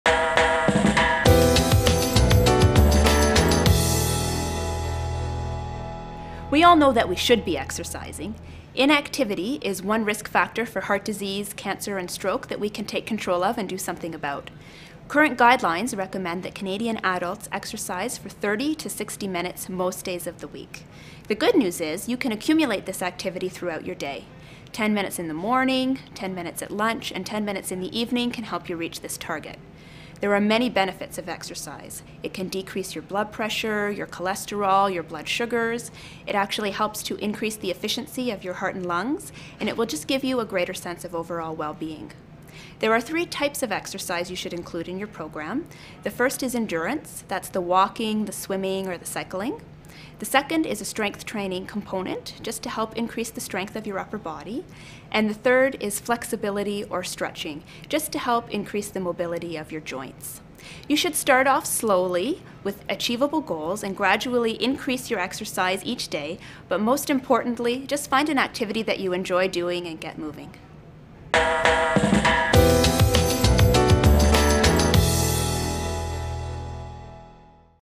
Exercise can help reduce your risk for heart disease, cancer and stroke (Health Q 013)
We all know we should be exercising; Inactivity is one risk factor for heart disease, cancer and stroke that we can take control of. Guidelines, and basics. Heather Haines - Kinesiologist - The credit Valley Hospital